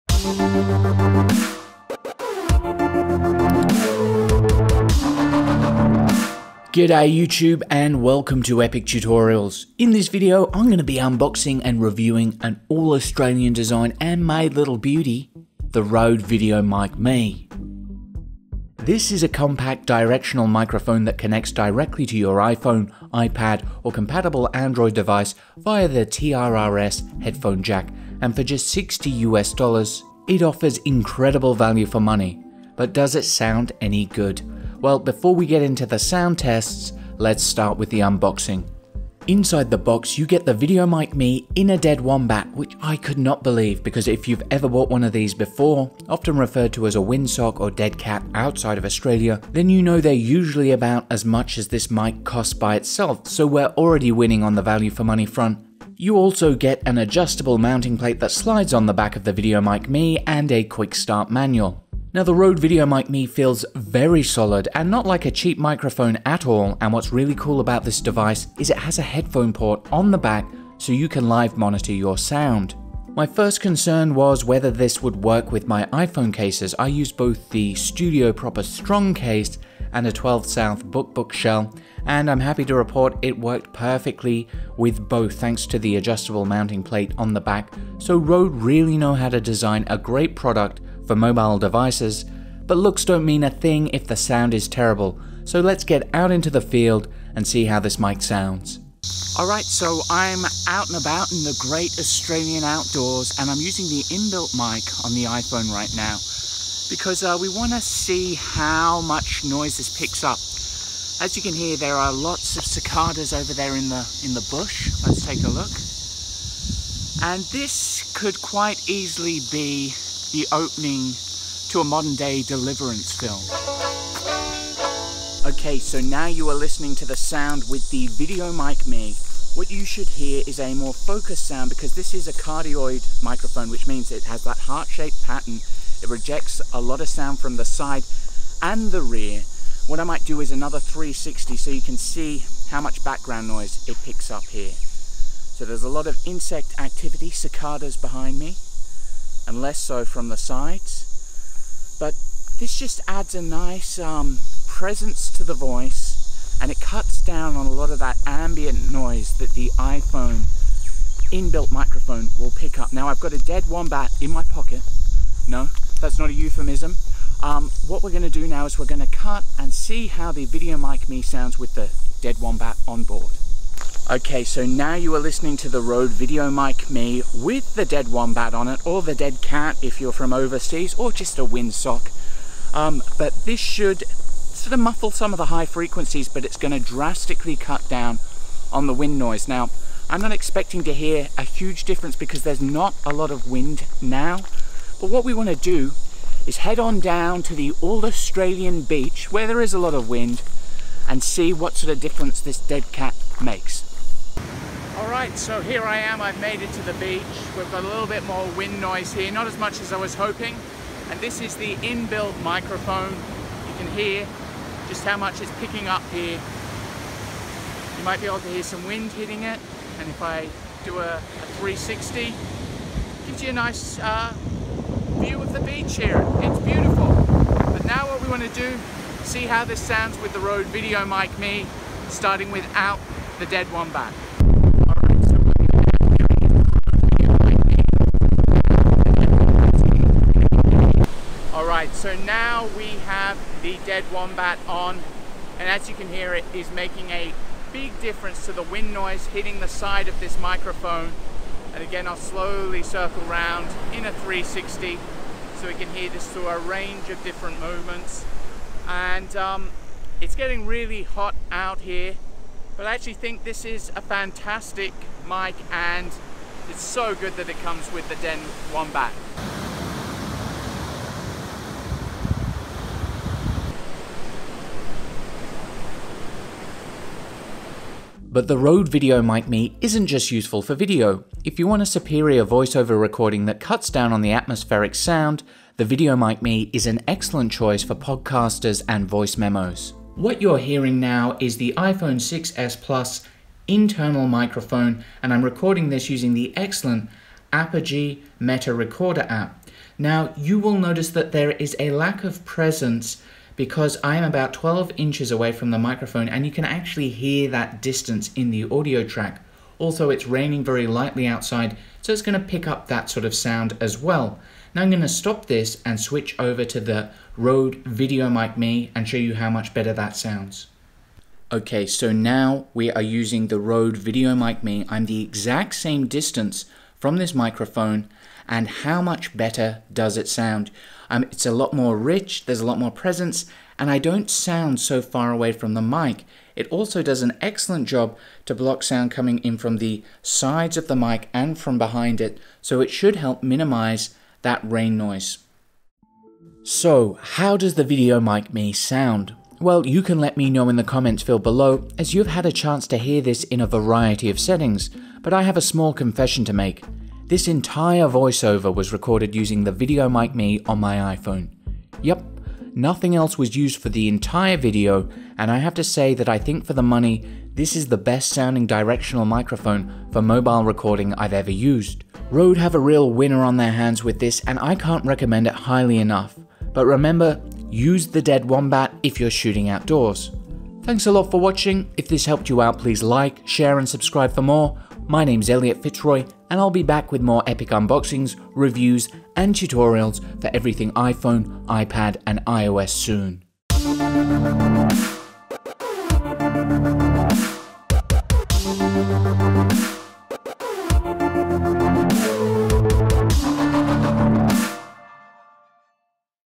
Epic Rode VideoMic Me Review - The Best iPhone Mic for Under $65!
Is the Rode VideoMic Me the best smartphone directional mic for under $65 USD? I think so, but hear for yourself in this epic review, unboxing and demo of Rode Microphones VideoMic Me for iPhone, iPad and Android!